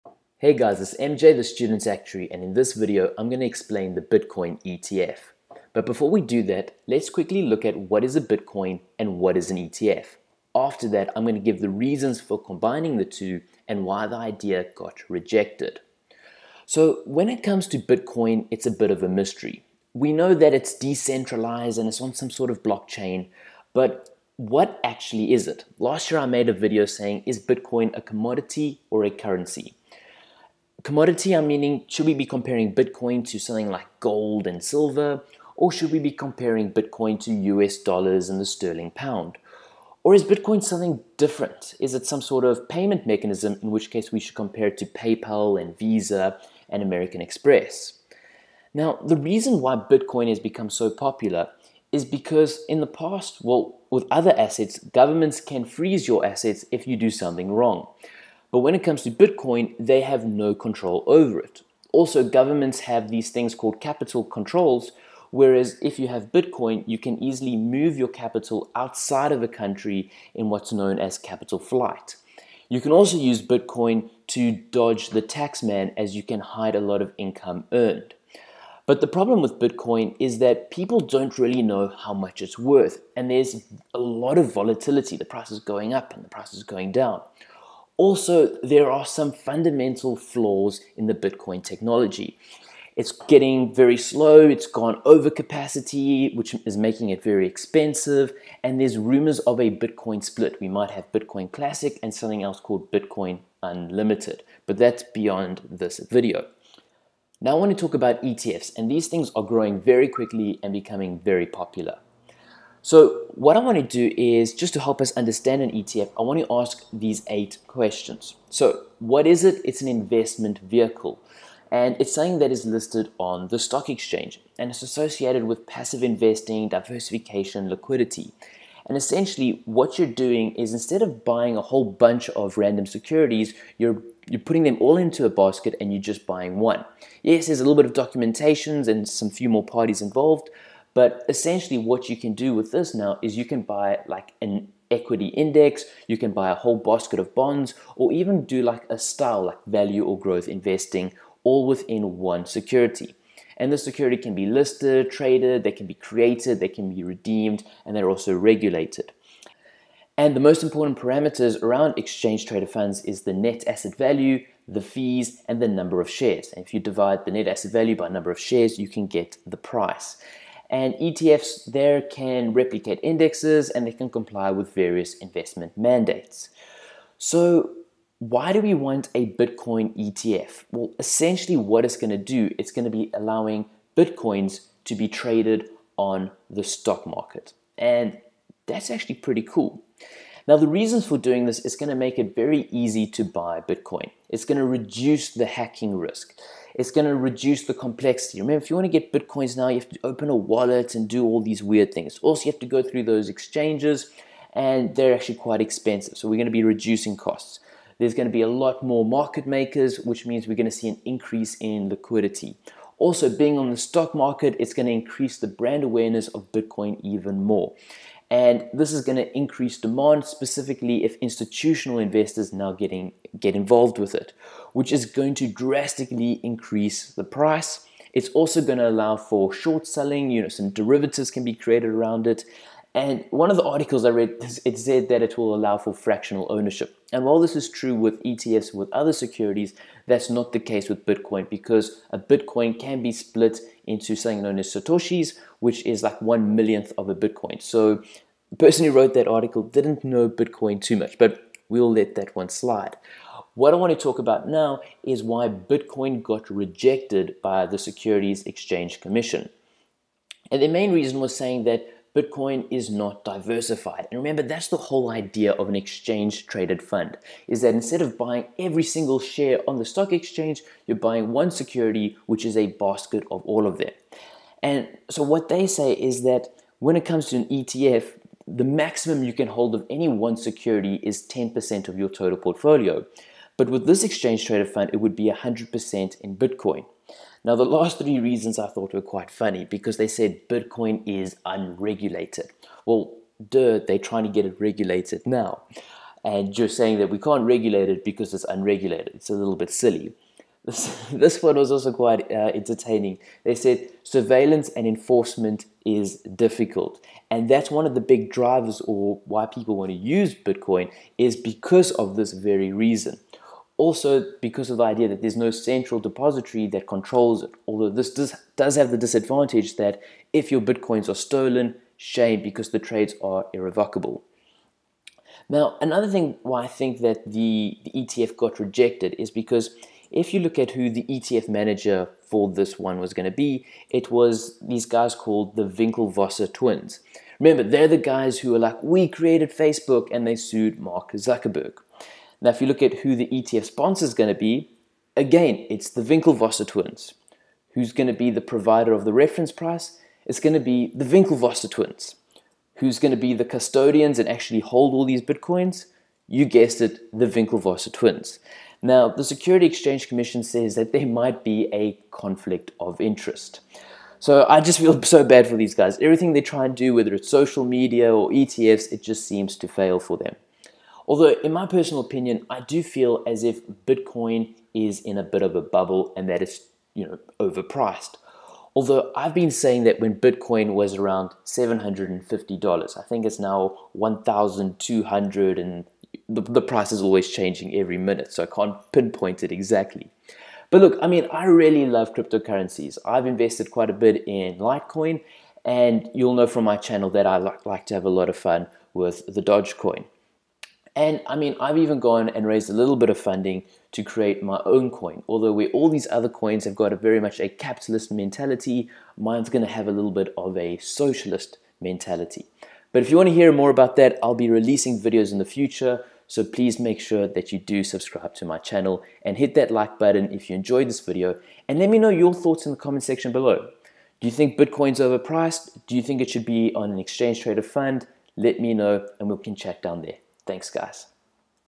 Bitcoin ETF Everything Explained
Big News at the moment is the Bitcoin ETF. What is it actually? What does it mean? How will it influence markets and prices? In this video I quickly look at Bitcoin, then I explain Exchange Traded Funds and then I discuss the reasons for putting Bitcoin on an ETF and then finally I explain why it got rejected by the Security Exchange Commission. Bitcoin to USD has been going crazy and it's all because of the possibility of the cryptocurrency being listed on the stock market. Hope you enjoy the video.